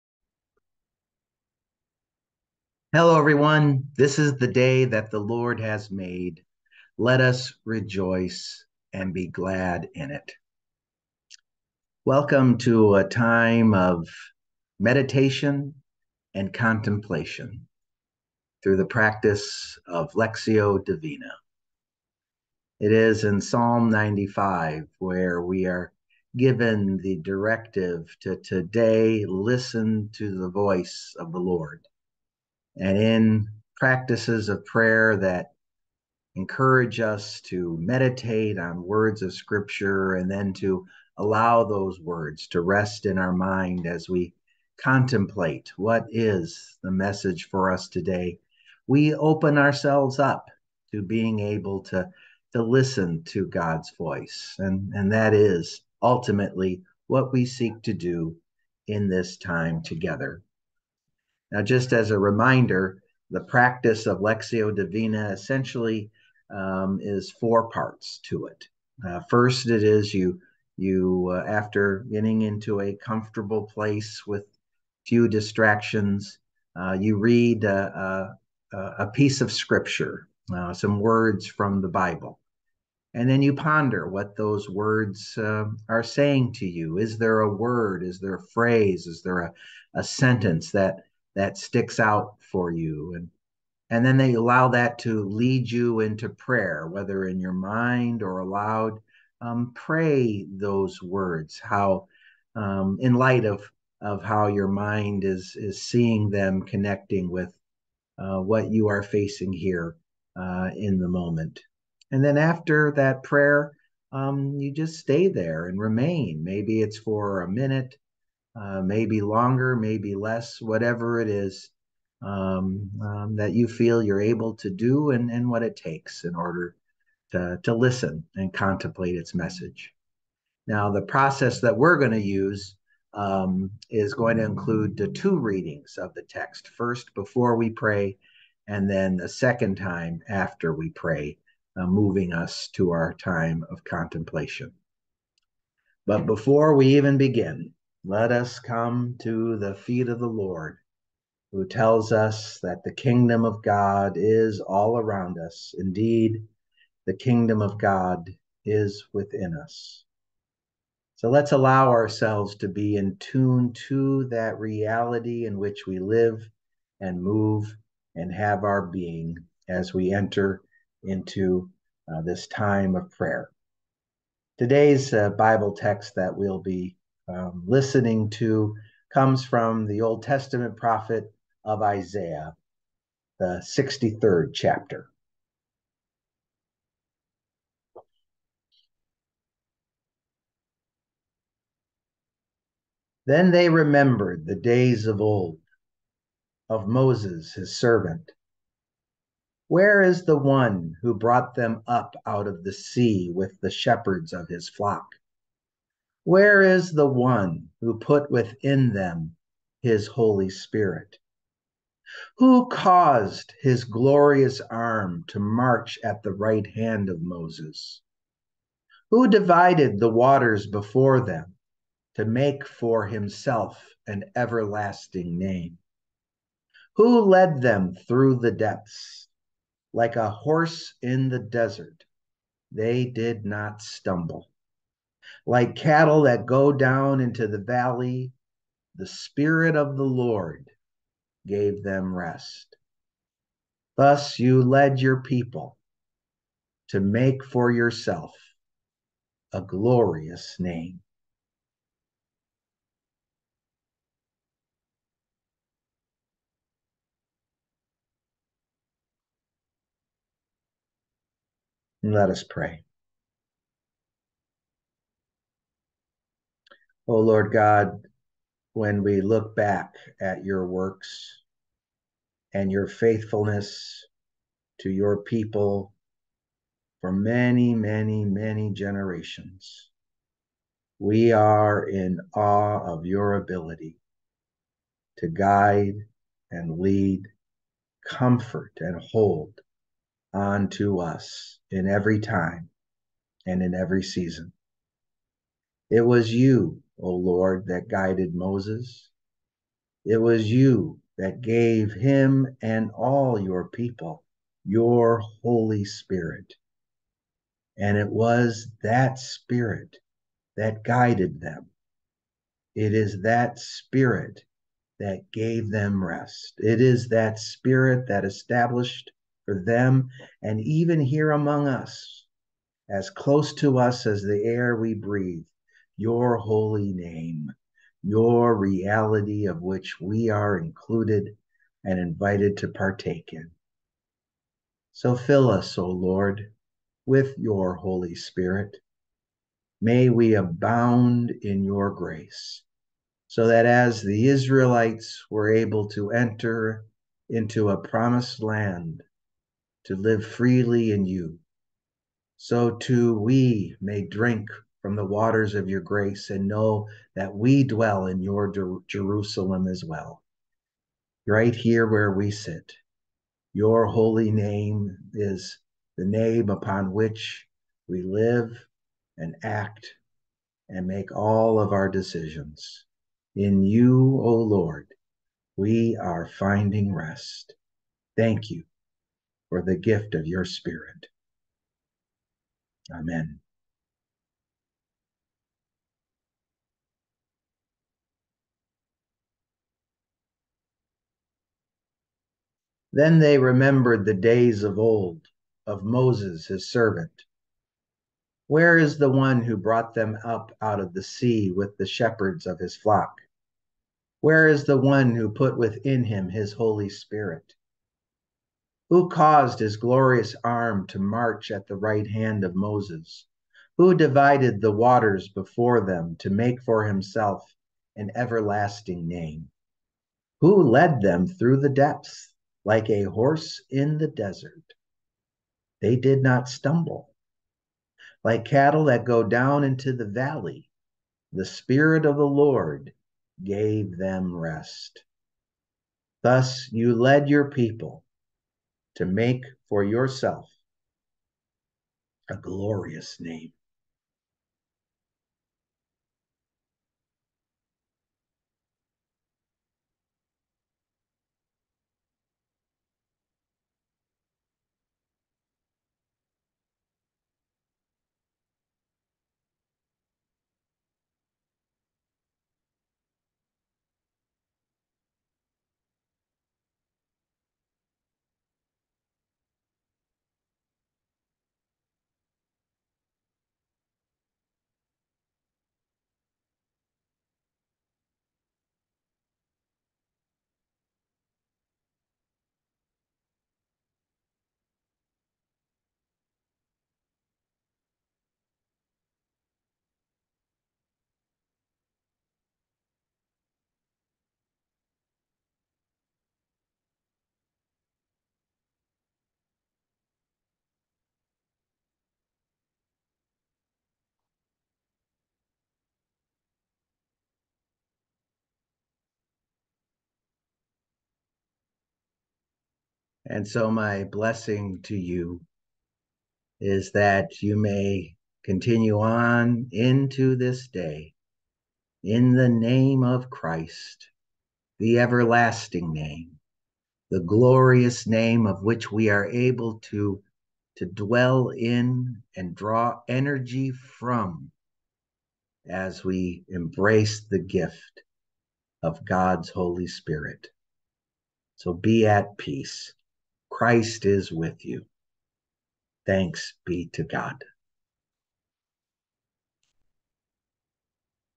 Contemplative Prayer - Lectio Divina
Lectio Divina
Isaiah 63:11-14
May 15, 2023
Pastor Tony Dusso
Our Savior's Lutheran Church, Beloit, WI